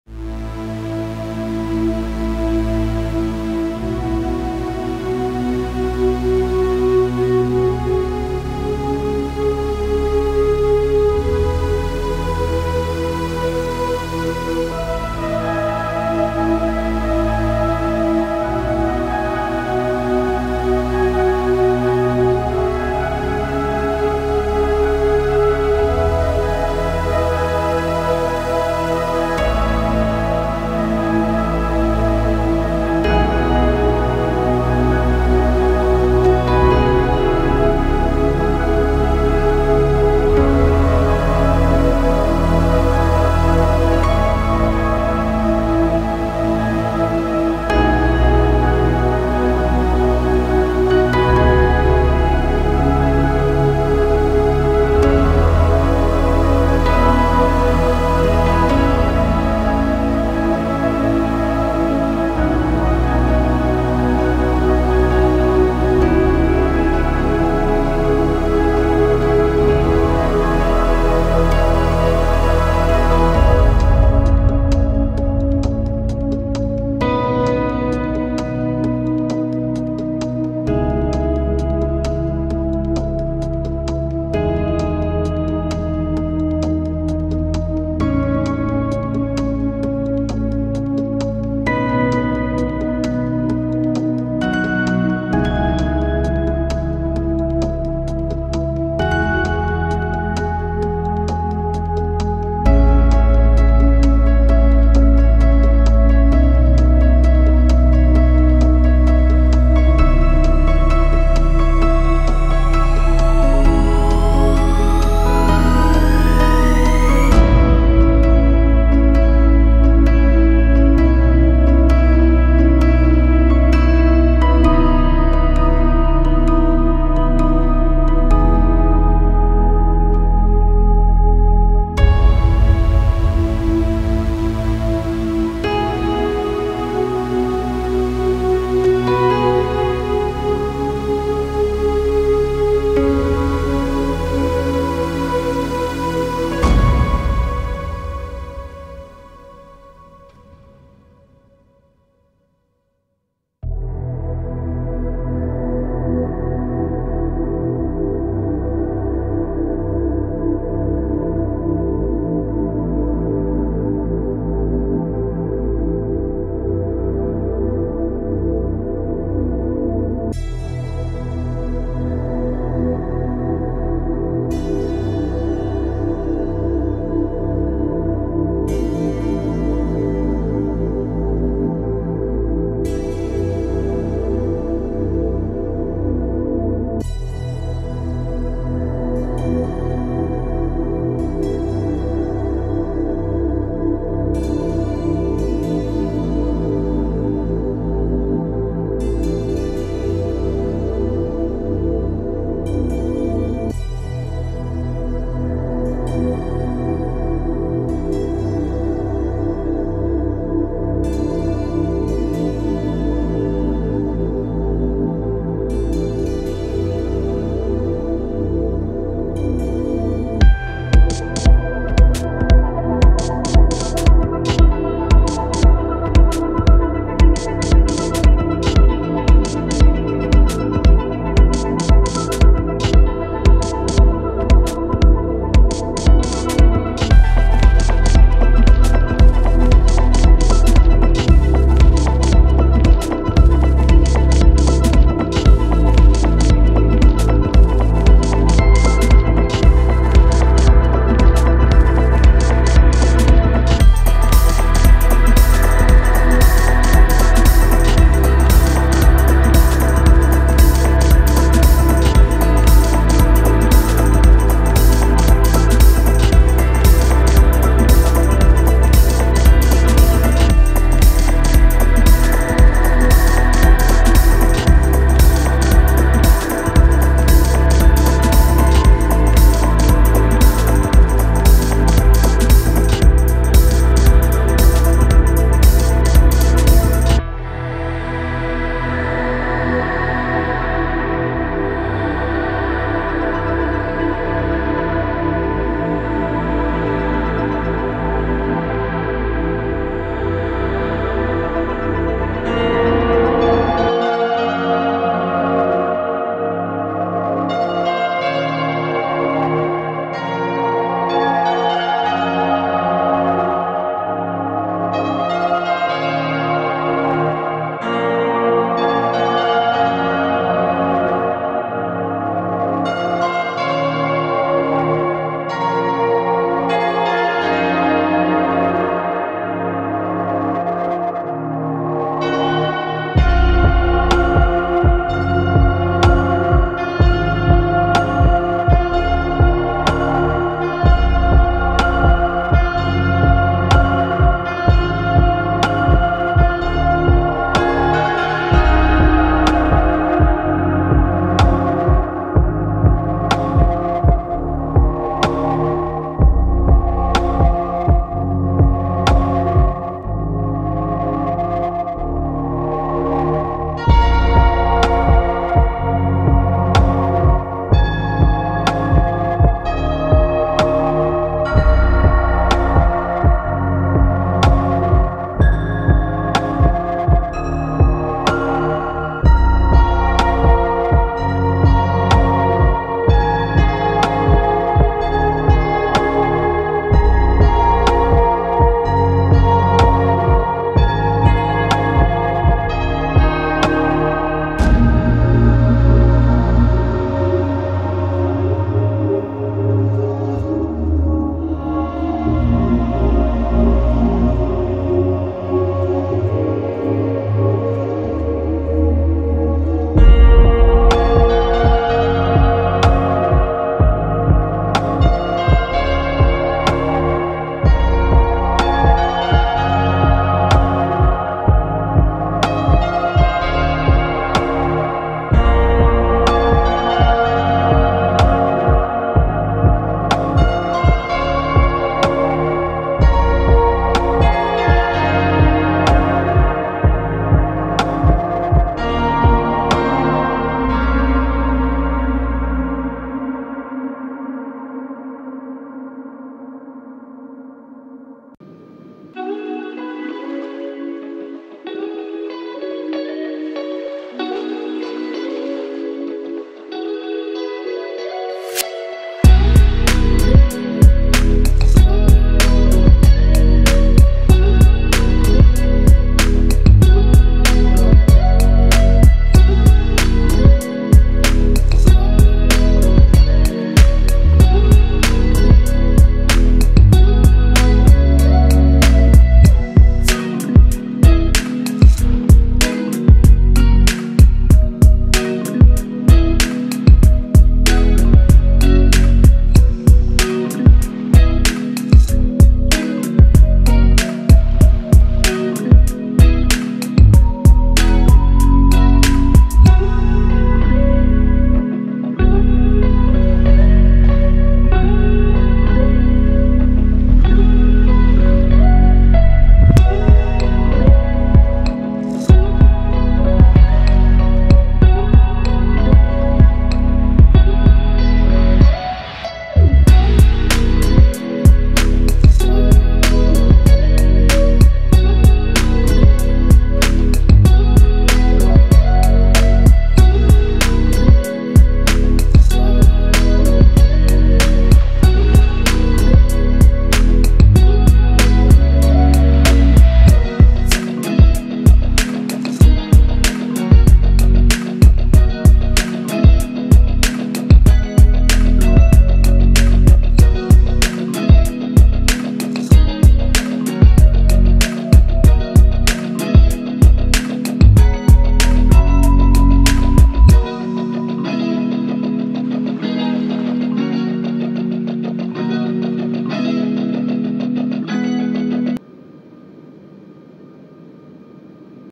Flight Window Music ! Chill Music ! Flight Mood Music
Flight Window Music en-route Canada International Airport

Feel the vibe of Travelling with your headphones on at your Home.